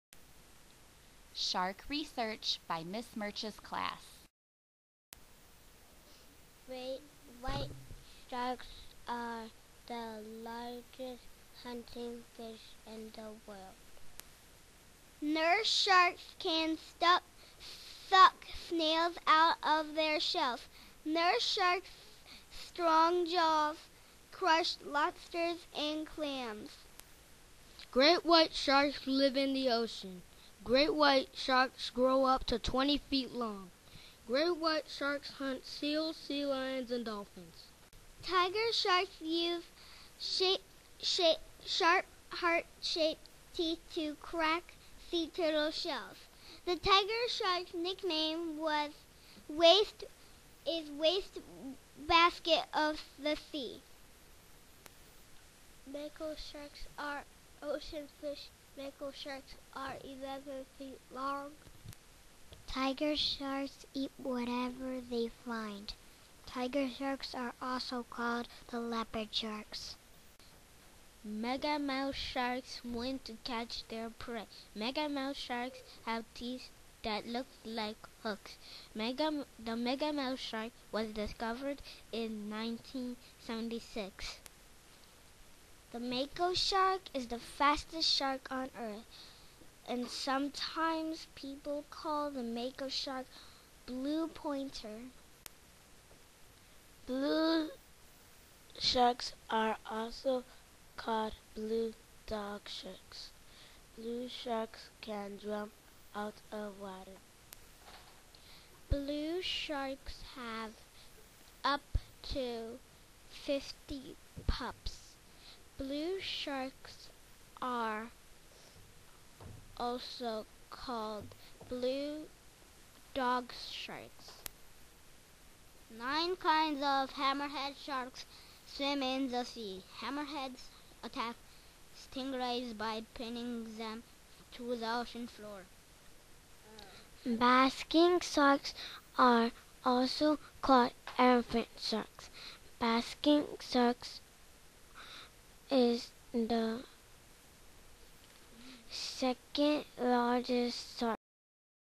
Shark Slideshow by Ms. Murch's Class 2012
Ms. Murch and her class did shark research.  They used Pebble Go and  the books from the Blazers:  Shark Zone series to find their facts.